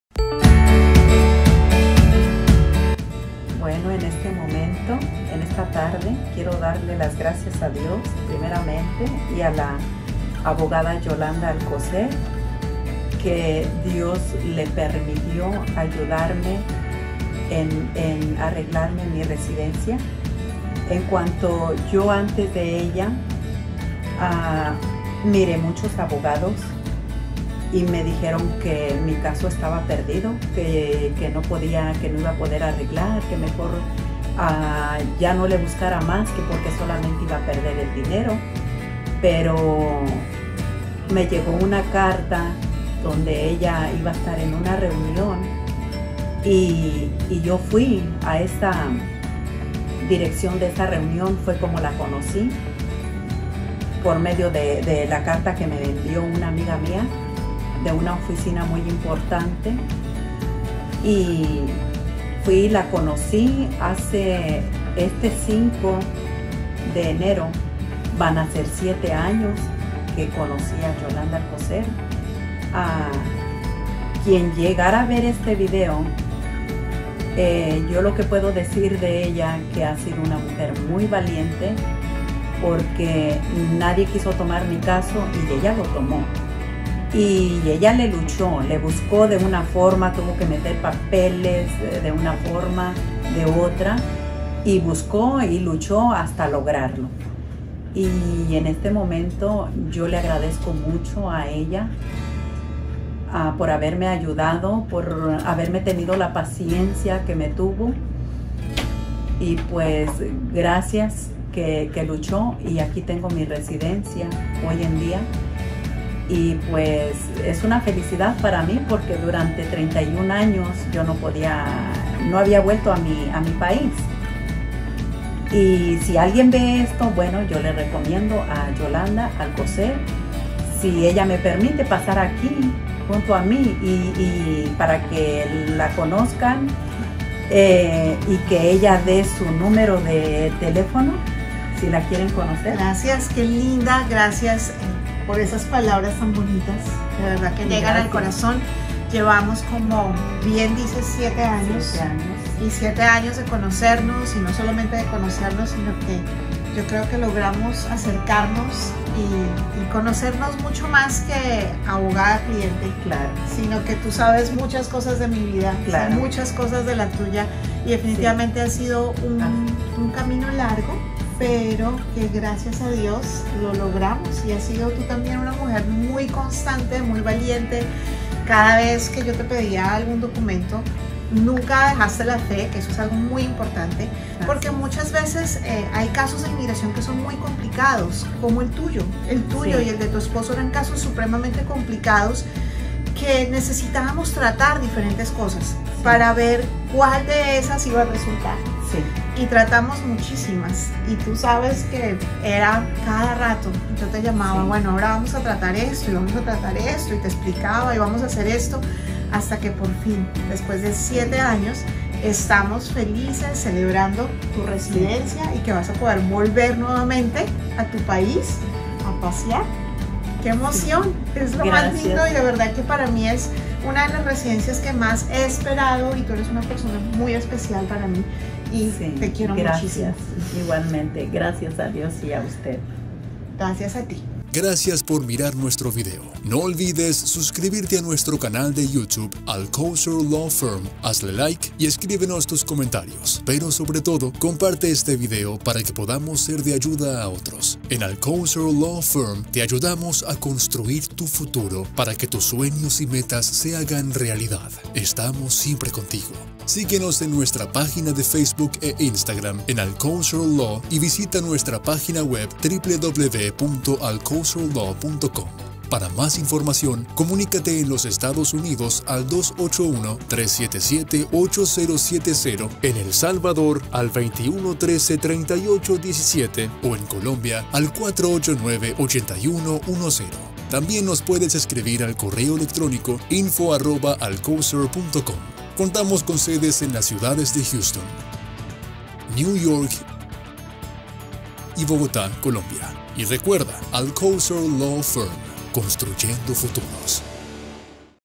Testimonio Matilde🥇 Inmigración a Estados Unidos
¡Estamos de celebración! Matilde obtuvo la residencia. Hoy es una nueva residente de los Estados Unidos y nos cuenta cómo fue su proceso y su experiencia durante el tiempo que llevamos su caso.

☎️ Contáctanos:
Teléfono Colombia: 489-8110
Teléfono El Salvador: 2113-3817